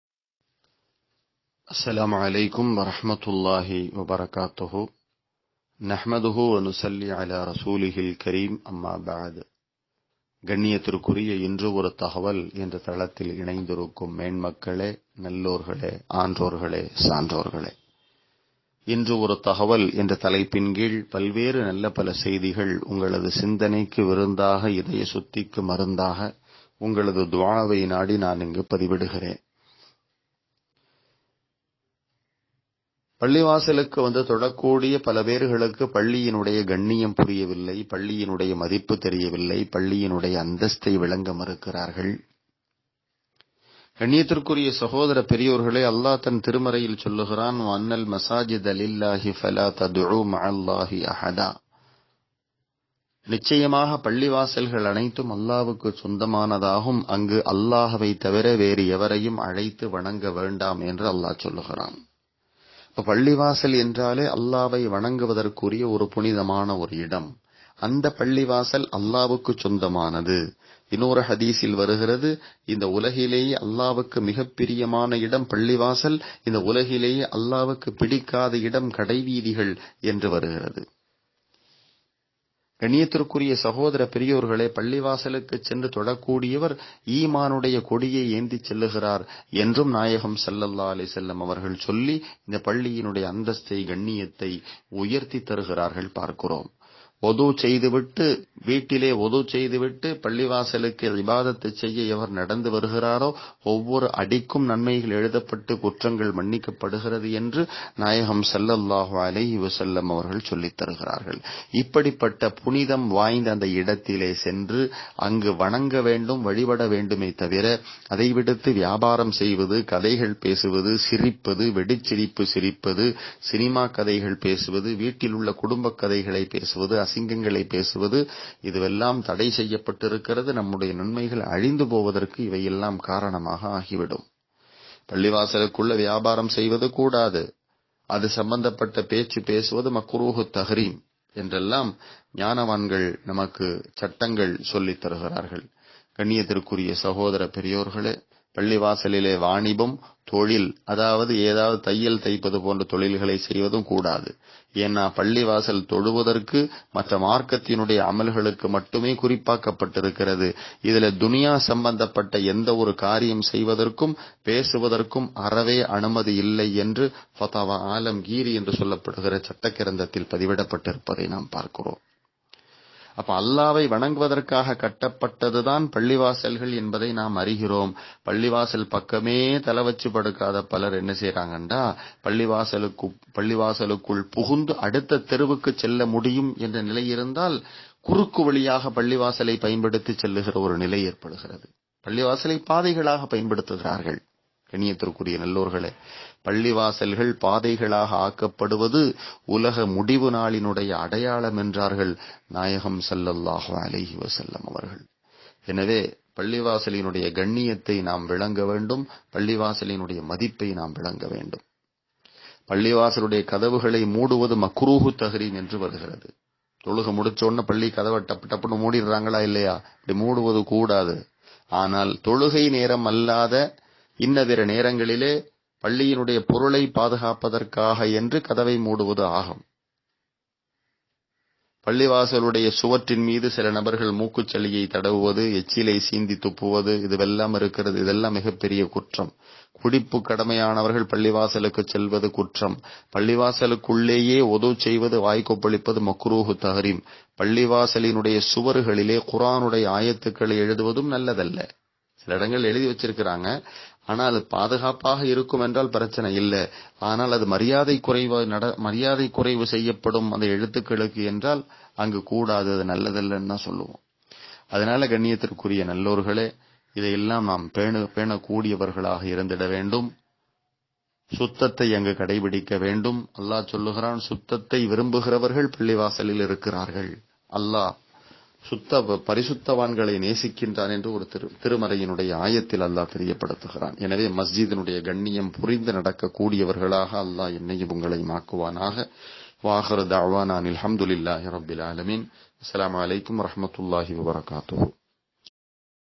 277 - ப்ளீஸ்... மஸ்ஜித்களின் கண்ணியம் காப்பீர்!!
🌸 *இன்று ஒரு தகவல்* ♻

📋 தலைப்பு:-
*ப்ளீஸ்... மஸ்ஜித்களின் கண்ணியம் காப்பீர்!!*

⏰ 4:55 Minutes

☪🎤 *மௌலவி*
*அப்துர் ரஹ்மான் ரஹ்மானி*

❇ வெளியீடு
*IOT CHENNAI*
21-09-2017